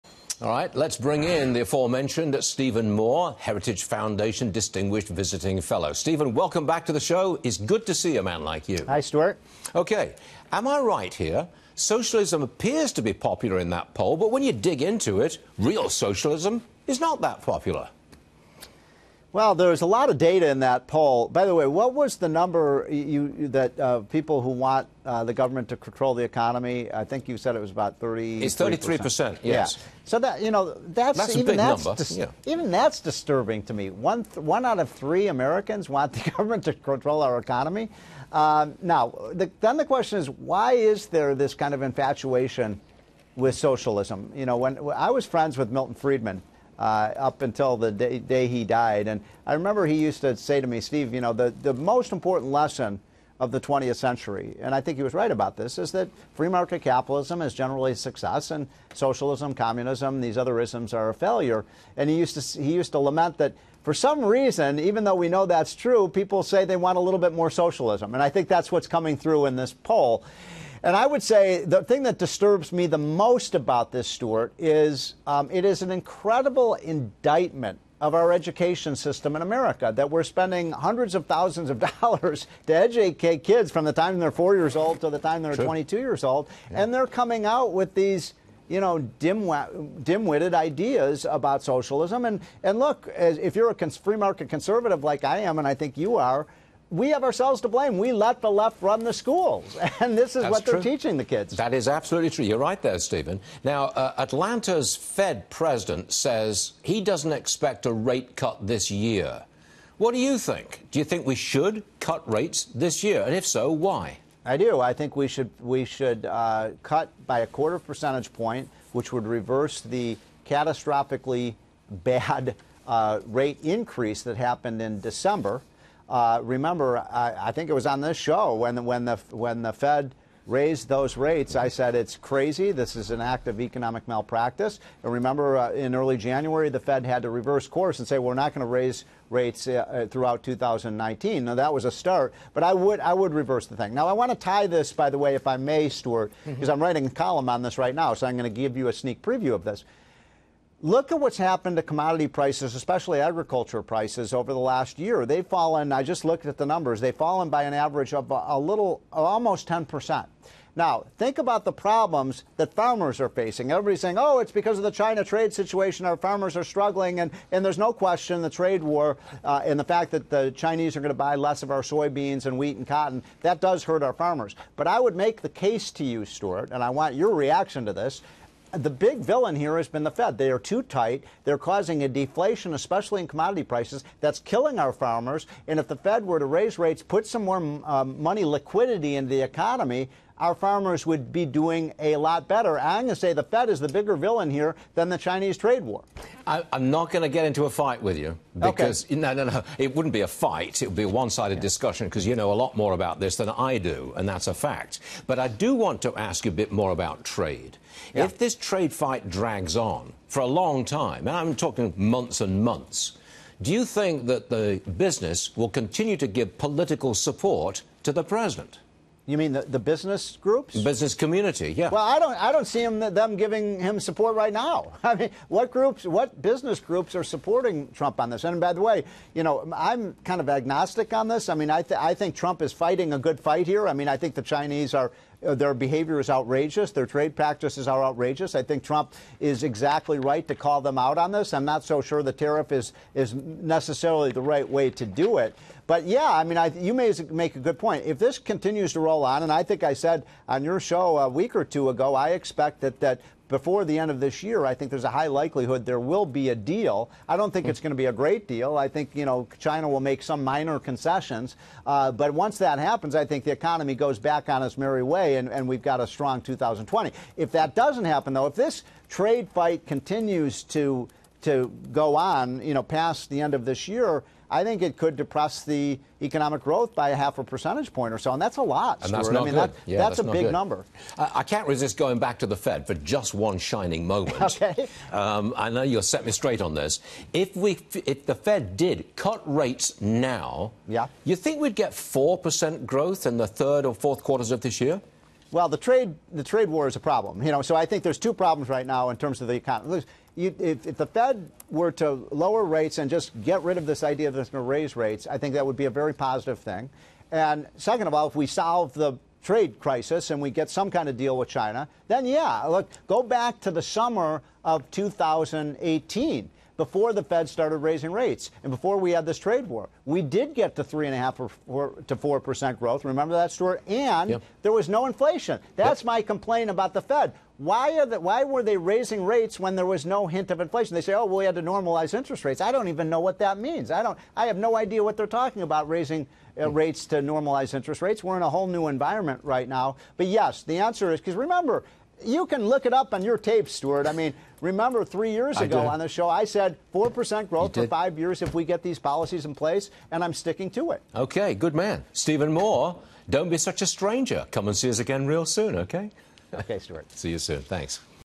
Stephen Moore calls for Fed rate cut this year
The Heritage Foundation's Stephen Moore on the rise of socialism and argues for a Federal Reserve interest rate cut this year.